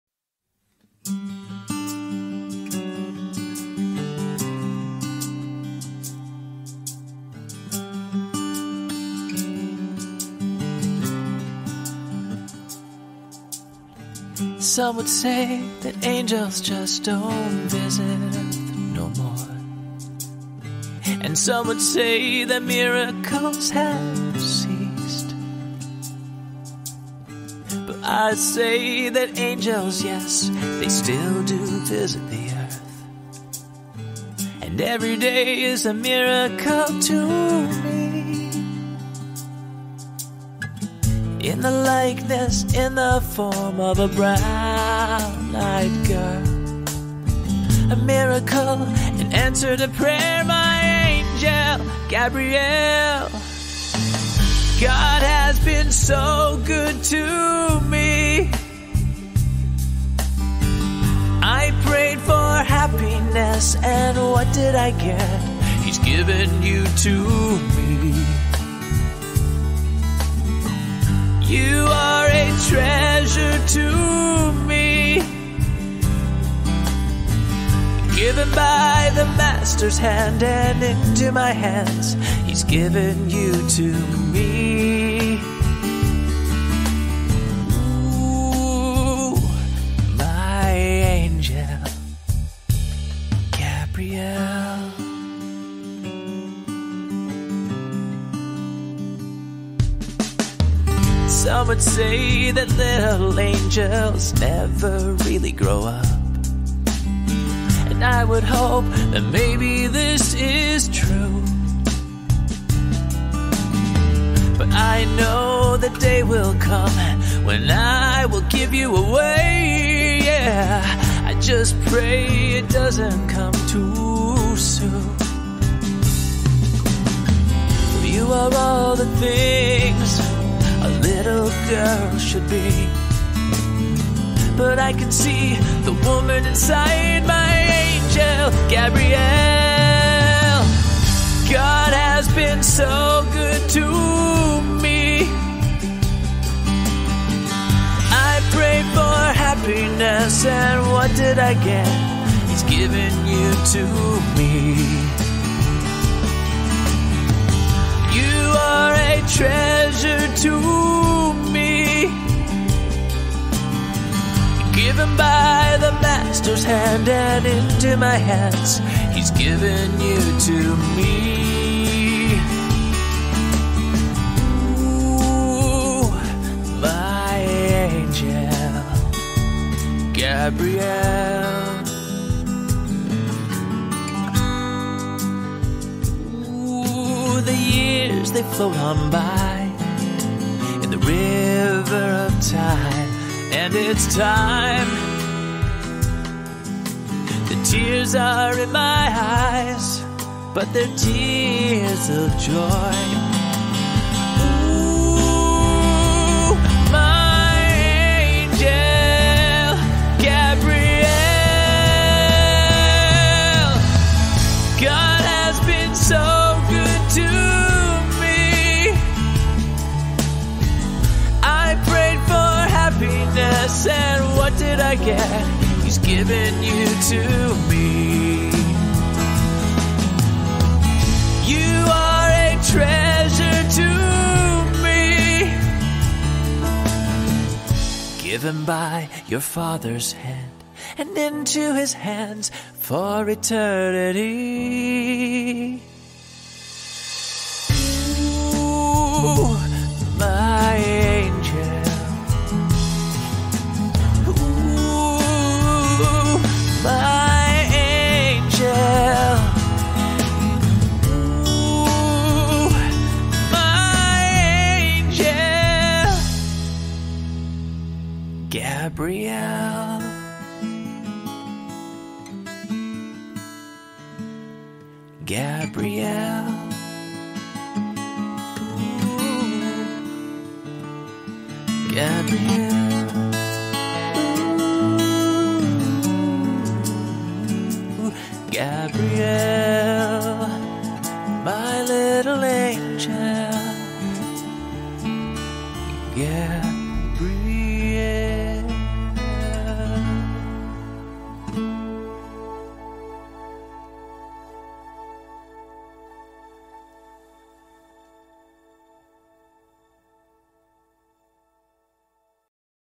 Happy 1st Anniversary Gabby and Brad!
Every night when my kids were little I sat at their bedside with my guitar and sang songs with them as they slowly drifted away into dreamland.  It was my absolute favourite part of the day, and I will forever cherish those bedtime memories.

I used to sing this song in particular to my daughter Gabrielle, which I wrote for her when she was very young.  One year ago today, I was honoured to sing it for her one more time at her wedding.  On May 29, 2021, Gabrielle married her sweetheart Brad Borsi.

As her Dad, I am so proud of my Gabrielle, for the amazing woman she has become, the Angel that she is to me and to everyone who knows her.  I am also proud of, and grateful to Brad - I couldn't ask for a better son-in-law.

Gabby and Brad, I congratulate you as today marks your first year of marriage.  The first of many, many more.  I love you both so much.  Happy 1st Anniversary!!!  Love, Dad.